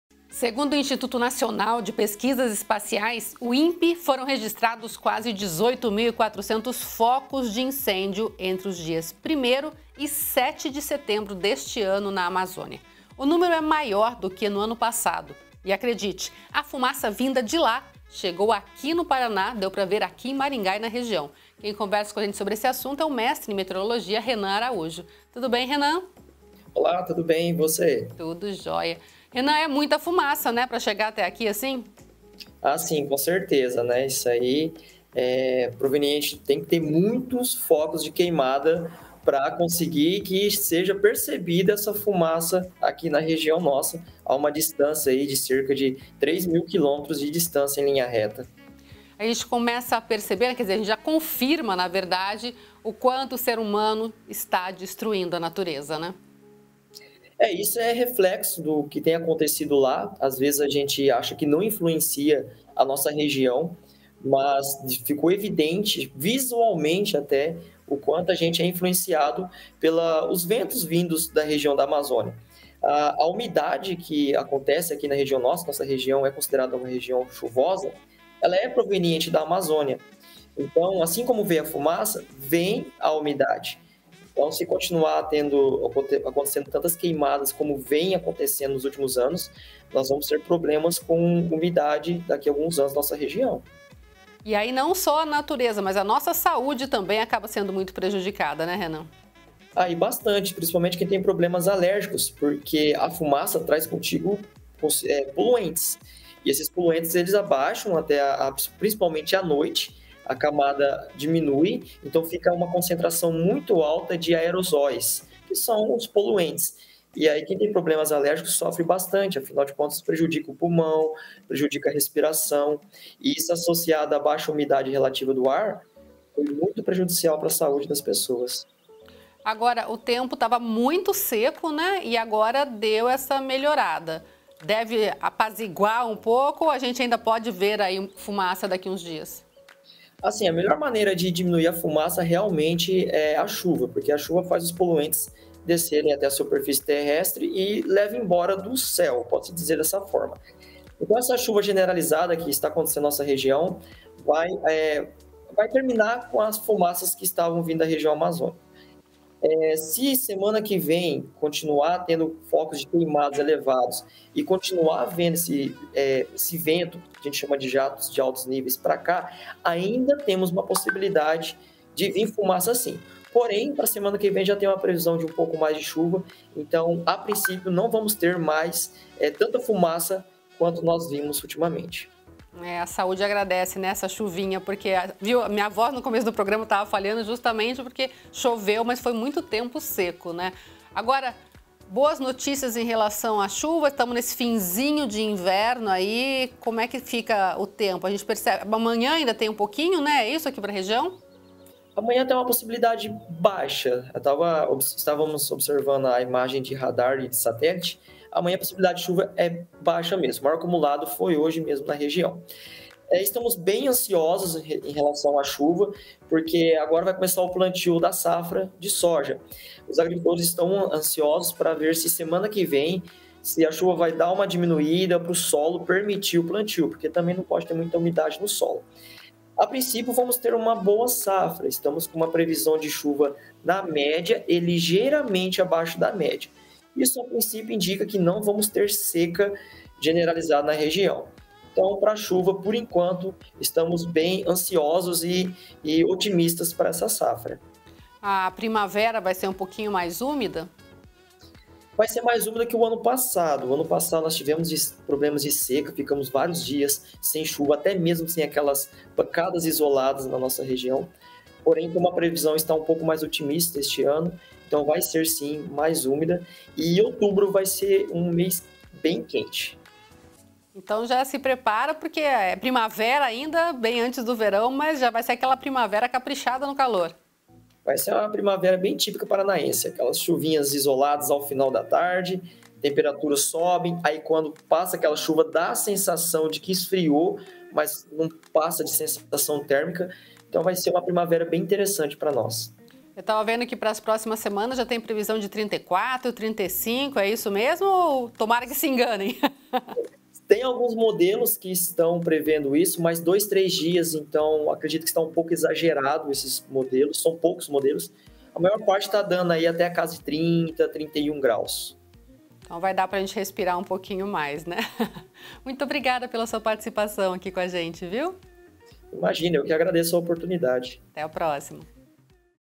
Fumaça de queimadas na Amazônia aparecem no norte do Paraná
Mestre em meteorologia explica como a fumaça chegou até Maringá e região - uma distância de quase 3 mil quilômetros